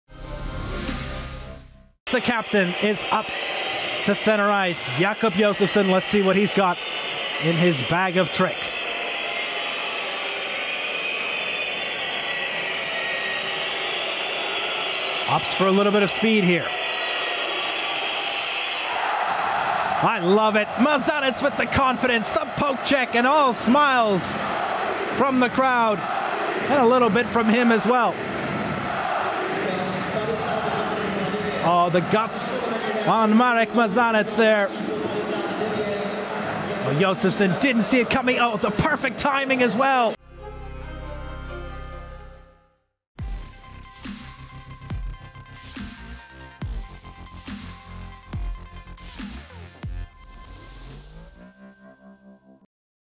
Mazanec with the well-timed Poke Check
Mountfield HK Goalkeeper Marek Mazanec denies Djurgården Stockholm Captain Jacob Josefson's penalty shot with a beautiful poke check.